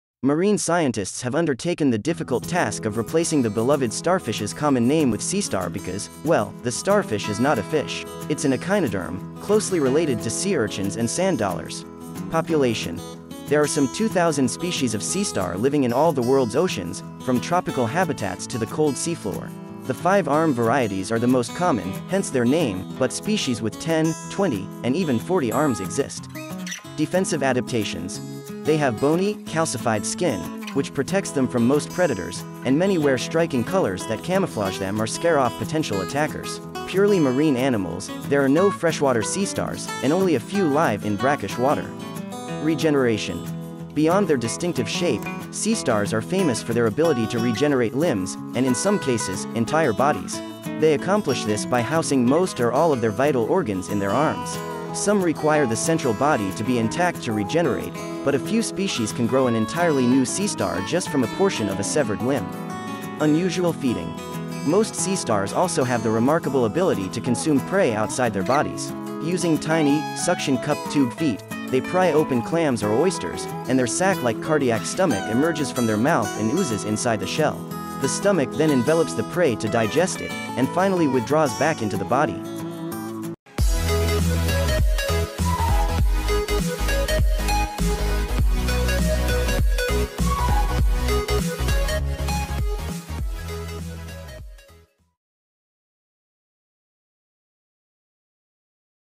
The Sea Star fish |Starfish care| - Unusual Feeding
The Sea Star |Starfish| - Unusual Feeding
Marine scientists have undertaken the difficult task of replacing the beloved starfish’s common name with sea star because, well, the starfish is not a fish. It’s an echinoderm, closely related to sea urchins and sand dollars...

Population.
There are some 2,000 species of sea star living in all the world’s oceans, from tropical habitats to the cold seafloor. The five-arm varieties are the most common, hence their name, but species with 10, 20, and even 40 arms exist.

Defensive Adaptations.
They have bony, calcified skin, which protects them from most predators, and many wear striking colors that camouflage them or scare off potential attackers. Purely marine animals, there are no freshwater sea stars, and only a few live in brackish water...

Regeneration.
Beyond their distinctive shape, sea stars are famous for their ability to regenerate limbs, and in some cases, entire bodies. They accomplish this by housing most or all of their vital organs in their arms. Some require the central body to be intact to regenerate, but a few species can grow an entirely new sea star just from a portion of a severed limb..

Unusual Feeding.
Most sea stars also have the remarkable ability to consume prey outside their bodies. Using tiny, suction-cupped tube feet, they pry open clams or oysters, and their sack-like cardiac stomach emerges from their mouth and oozes inside the shell. The stomach then envelops the prey to digest it, and finally withdraws back into the body.